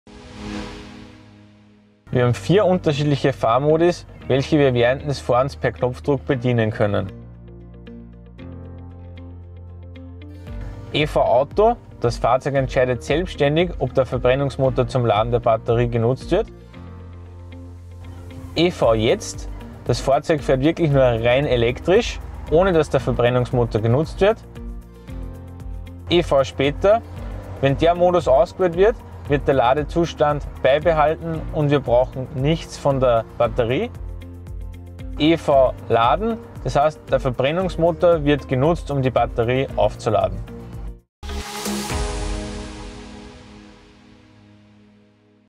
Verschiedene Fahrmodi | Ford Modelle | Ford Danner
...was ist eigentlich alles möglich? Probiern Sie es aus in unserem Haus, egal was Sie bevorzugen wir haben für Sie die perfekte Lösung :)
ford modelle

Perfekt abgestimmt

Der selektive Fahrmodus-Schalter ermöglicht eine schnelle und einfache Auswahl unterschiedlicher Fahrmodi, um die Fahreigenschaften individuell Ihren Bedürfnissen anzupassen. Je nach gewähltem Modus werden verschiedene Faktoren des Getriebes beeinflusst.